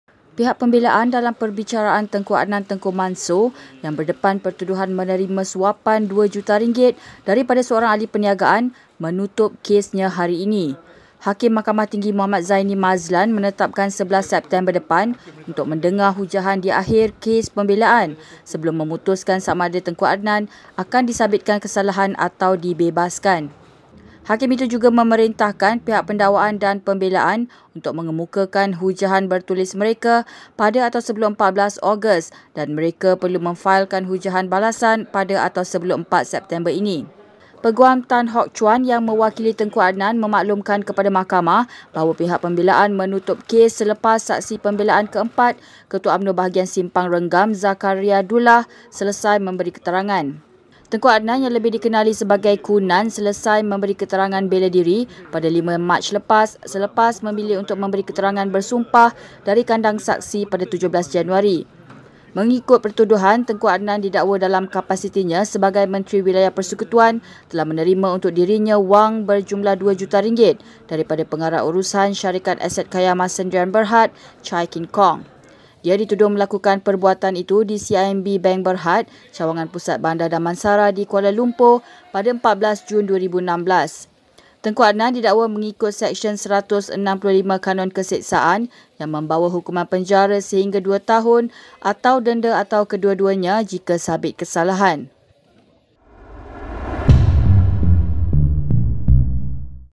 Perbicaraan rasuah Tengku Adnan: Pembelaan tutup kes
Pihak pembelaan dalam perbicaraan Tengku Adnan Tengku Mansor yang berdepan pertuduhan menerima suapan RM2 juta daripada seorang ahli perniagaan, menutup kesnya, hari ini.

Hakim Mahkamah Tinggi Mohamed Zaini Mazlan menetapkan 11 September depan untuk mendengar hujahan di akhir kes pembelaan sebelum memutuskan sama ada beliau akan disabitkan kesalahan atau dibebaskan.